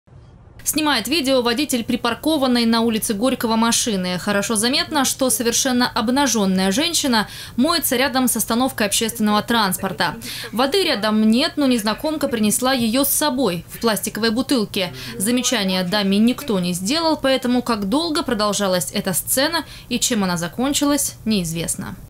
В Сочи голая женщина устроила водные процедуры в самом центре Сочи. Новости Эфкате
Снимает видео водитель припаркованной на улице Горького машины. Хорошо заметно, что совершенно обнаженная женщина моется рядом с остановкой общественного транспорта. Воды рядом нет, но незнакомка принесла ее с собой в пластиковой бутылке. Замечание даме никто не сделал, поэтому как долго продолжалась эта сцена и чем она закончилась- неизвестно.

Сочи. Новости.
Телеканал Эфкате.